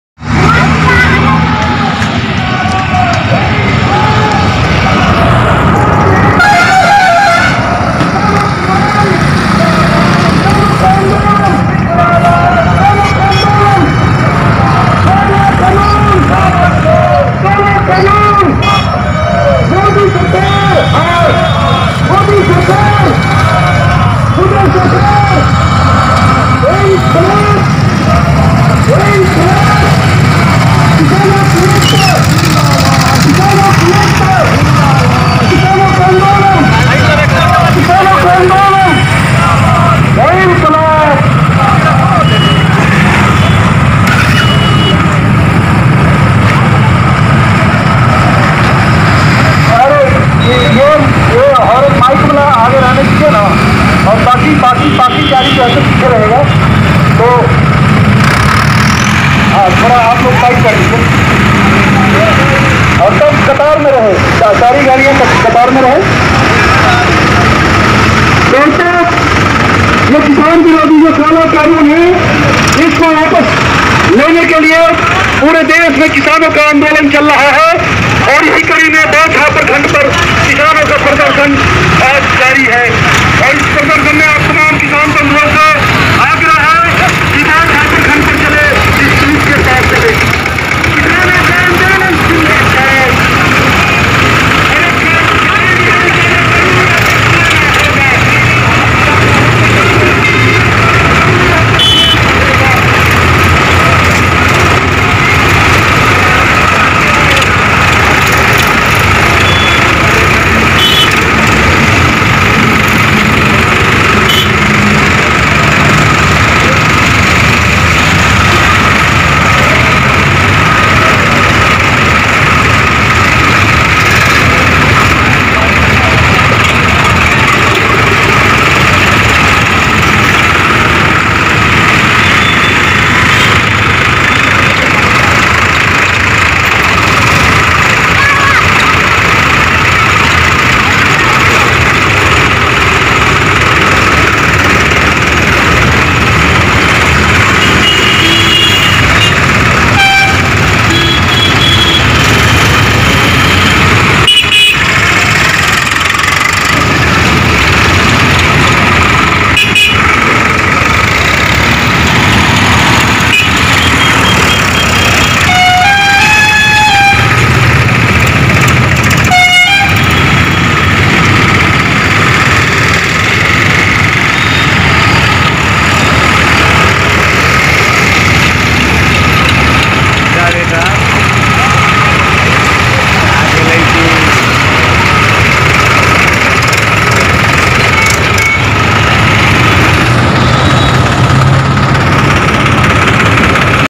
स्पॉटlive:मुजफ्फरपुर के बोचहां में महागठबंधन की ओर से किसान बचाओ ट्रैक्टर रैली एवं प्रदर्शन!
स्पॉटliveरिपोर्ट!